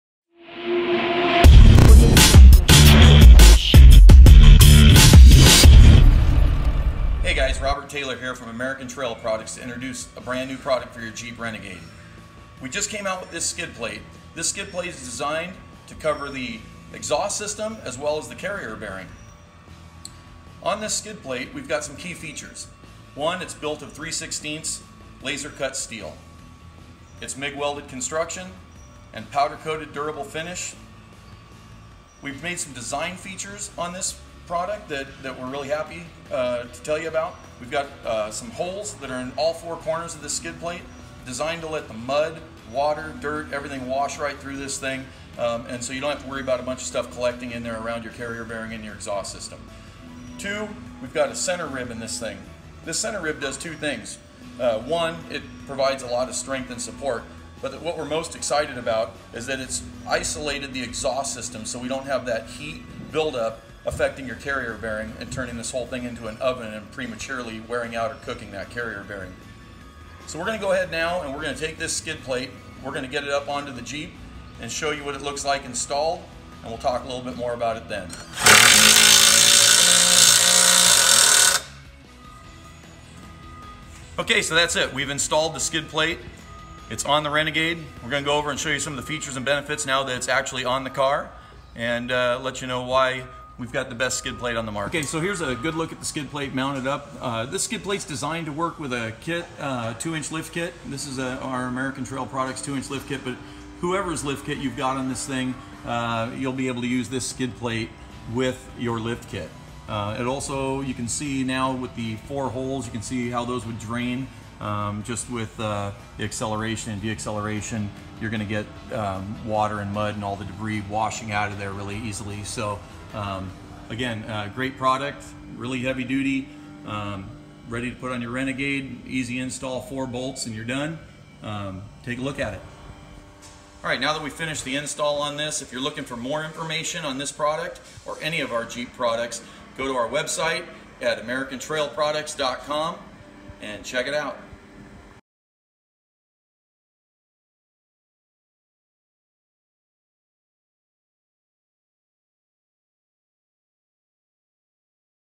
American Trail Products Jeep Renegade Carrier Bearing Skid Plate
ATP carrier bearing skid plate.. don’t cook 🔥 your carrier bearing make sure your skid plate has a heat shield to deflect the heat from your exhaust away from your bearing ! All of our products are made here in the USA! 🇺🇸